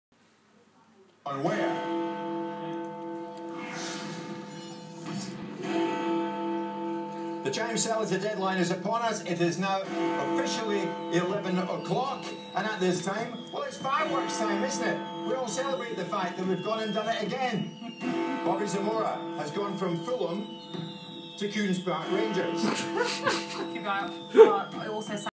What did he just call Queens Park Rangers .... Jim White yo
Sky presenter had a slip Of the tongue when Cisse comes on the screen and inadvertently calls Queens Park Rangers Coons Park Rangers